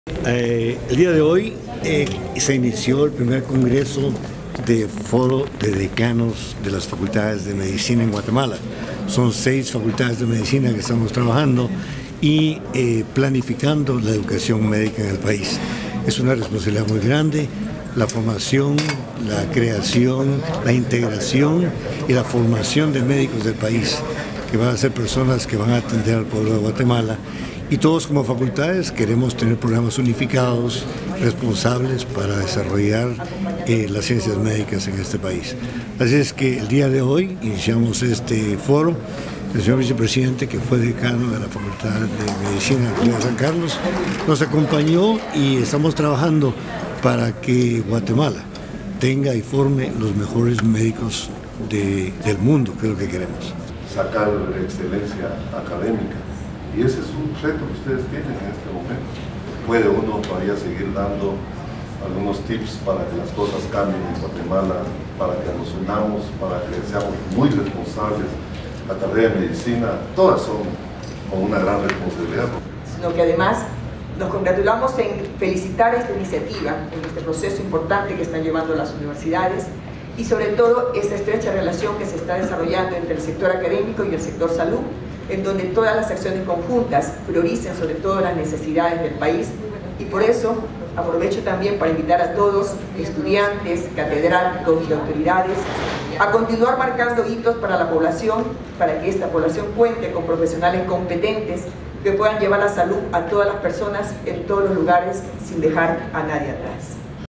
Vicepresidente y decanos de facultades de Medicina revisan modelo educativo de escuelas médicas
Seis facultades de Medicina se reunieron hoy para revisar y planificar la educación médica en el país, así como marcar una ruta hacia la acreditación académica de dichas casas de estudio, discusión en la que participó el Vicepresidente de la República, Jafeth Cabrera, dada su profesión de médico.

El evento, I Congreso de Facultades de Medicina 2018, fue inaugurado, j unto a Cabrera, por los decanos de Medicina de la Universidades de San Carlos, Rafael Landívar, Mariano Gálvez, Francisco Marroquín, Da Vinci y Mesoamericana.

Los galenos citados estarán dialogando hoy sobre los estándares del proceso de acreditación  de las facultades de Medicina a nivel internacional, y su situación actual a nivel latinoamericano; Ruta y crítica para el inicio y finalización del proceso de acreditación; Conferencia Excelencia y Valores; y Situación actual de los estudiantes de medicina en su práctica médica Hospitalaria.

Imperativo garantizar una educación médica de calidad, dice vicegobernante

Cabrera destacó que el camino hacia la acreditación de los estudios de Medicina en el país ha sido un proceso “complicado”, pero es imperativa la evaluación de los mismos para poder obtenerla y garantizar una educación médica de calidad.

Además, comentó que es necesario revisar cómo se involucran los médicos externos, internos y residentes a la actividad hospitalaria, ya “que tienen una gran responsabilidad en sus manos, la salud del pueblo".

Durante la actividad, el actual decano de Ciencias Médicas la Universidad Mariano Gálvez y  ex vicepresidente de la República, Rafael Espada, señaló la responsabilidad de la formación de los profesionales que atenderán la salud de los guatemaltecos y la relevancia que esta cobra en el referido cónclave.

“Es una responsabilidad muy grande la creación, integración y formación de médicos del país, que serán personas que atenderán al pueblo de Guatemala. Todos, como facultades, queremos programas unificados, responsables para desarrollar las ciencias médicas en el país”, puntualizó Espada.

*Con información de Selvyn Curruchich/DCA.